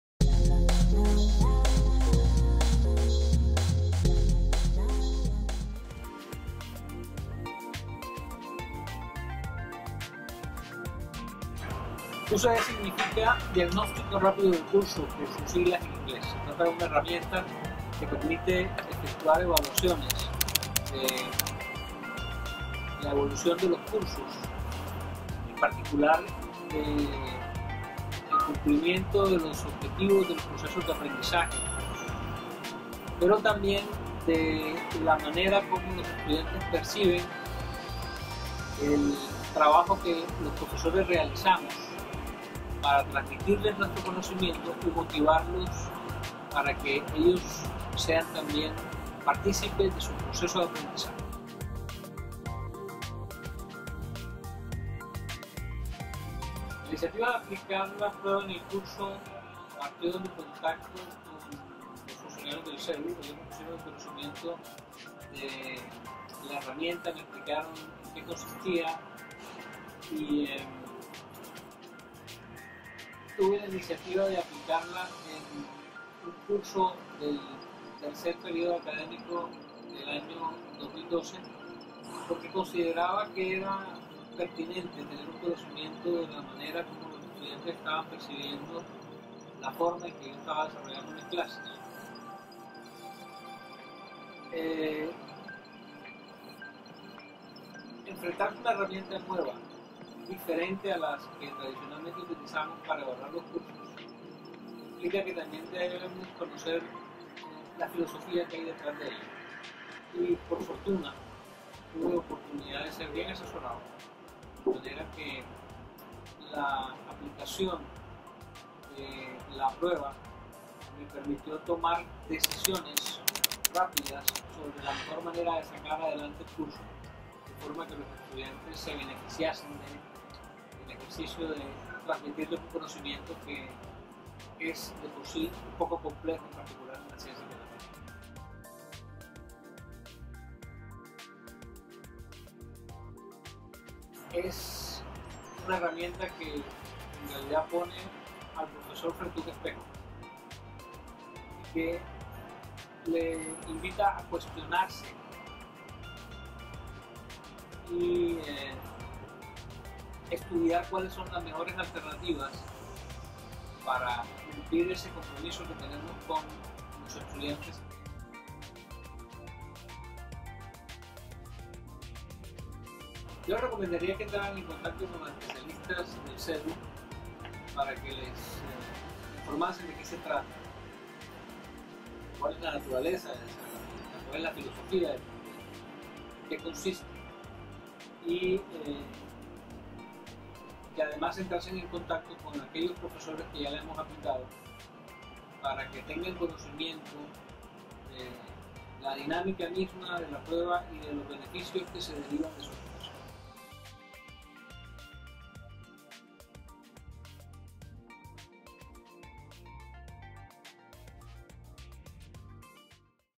Quick Course Diagnosis (QCD)
El diagnóstico rápido de curso es una estrategia creada por la docente norteaméricana, Barbara Millis, de la Universidad de Texas, que permite conocer la percepción de los estudiantes sobre la clase, en cualquier punto del semestre. El CEDU de Uninorte, hizo la adaptación de los instrumentos y además diseñó una aplicación digital de la evaluación que optimiza la gestión de la información.